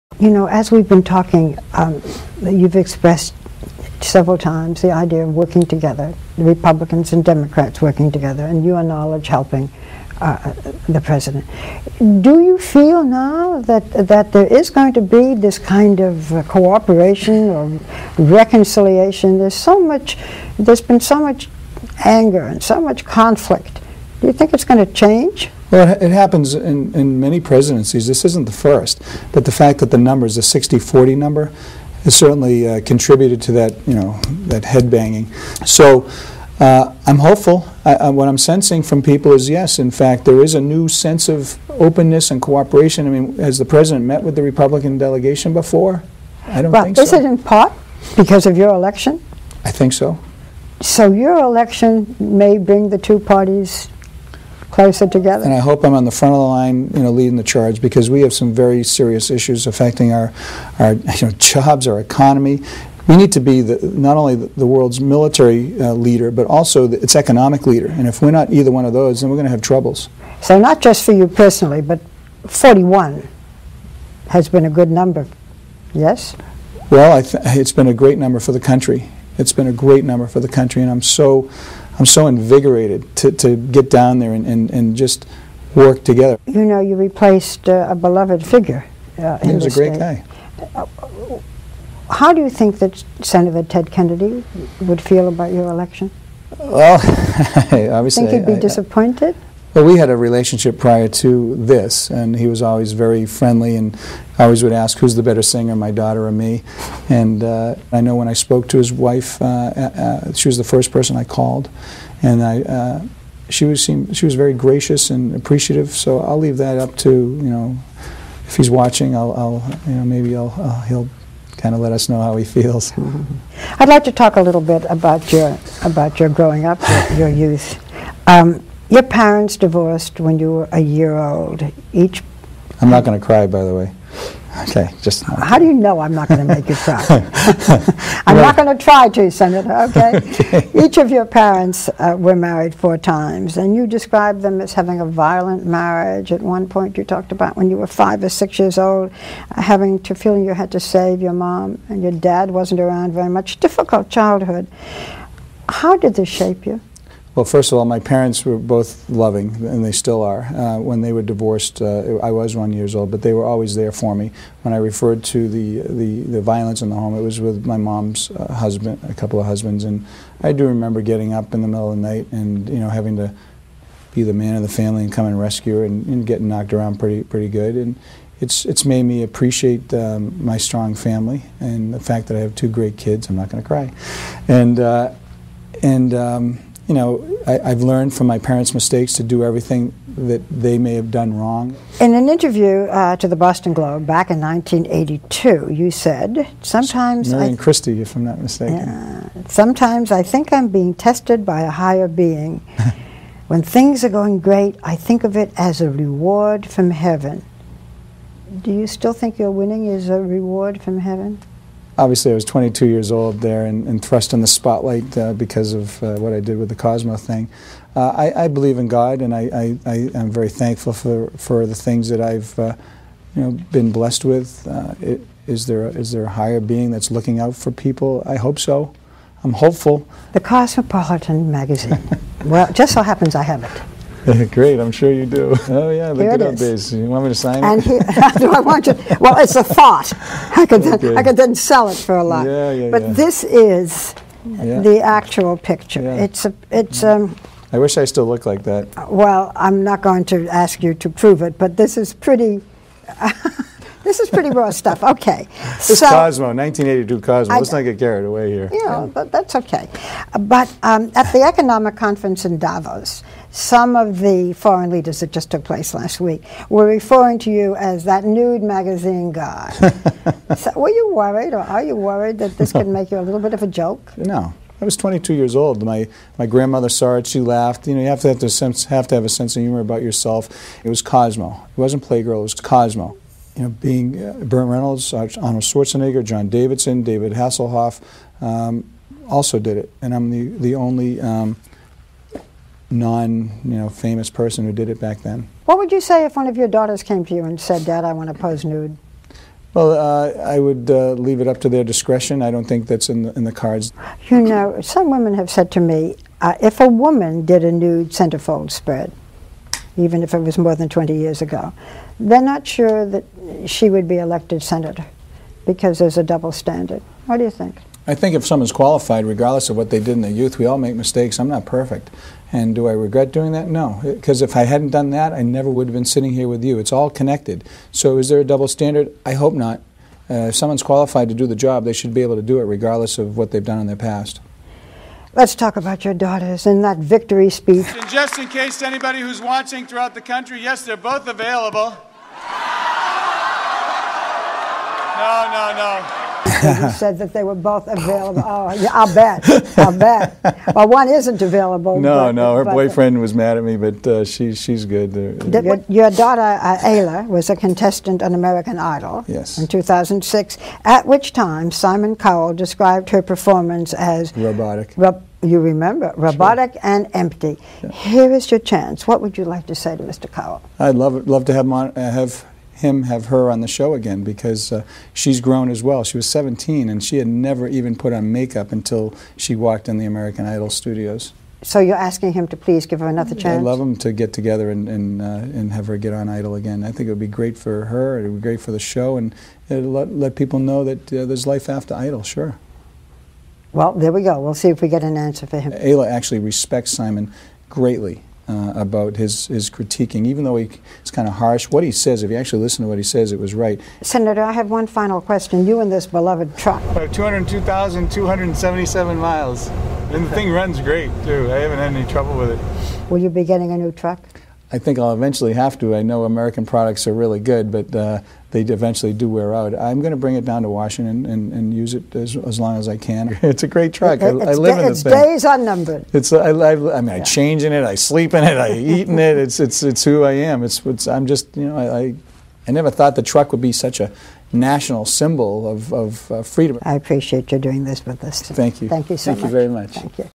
Barbara Walters Exclusive: Scott Brown Full Interview Part 3 of 3
Barbara Walters sits down with Senator Elect Scott Brown and talks about how he was able to win and how he plans to reform Washington. He also talks about how he plans to bring both Democrats and Republicans together.

Video Courtesy of ABCNEWS.COM